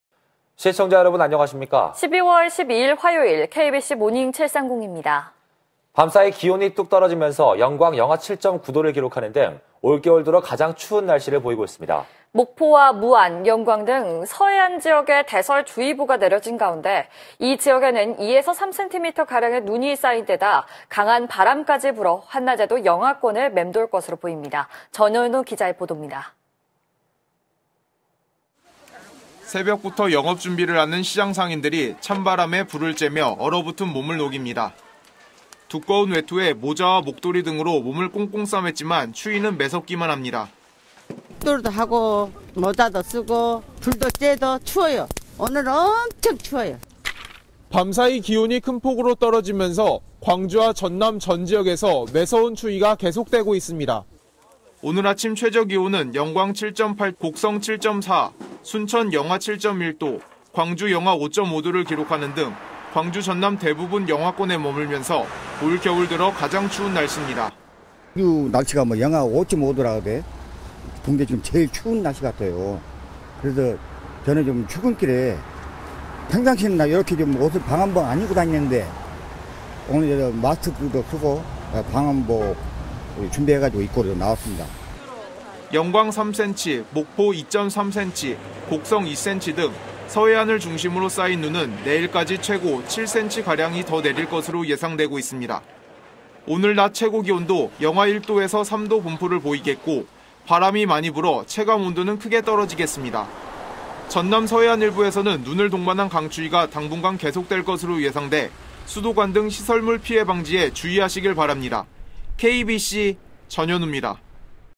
올 들어 가장 추운 날씨...일부 지역 눈
밤 사이 기온이 뚝 떨어져 영광 영하 7.9도를 기록하는 등 올 겨울 들어 가장 추운 날씨를 보이고 있습니다.

목포와 무안, 영광 등 서해안 지역에 대설주의보가 내려진 가운데 이 지역에는 2-3cm 가량의 눈까지 쌓인데다 강한 바람까지 불어 한낮에도 영하권을 맴돌 것으로 보입니다.

전현우 기자의 보돕니다.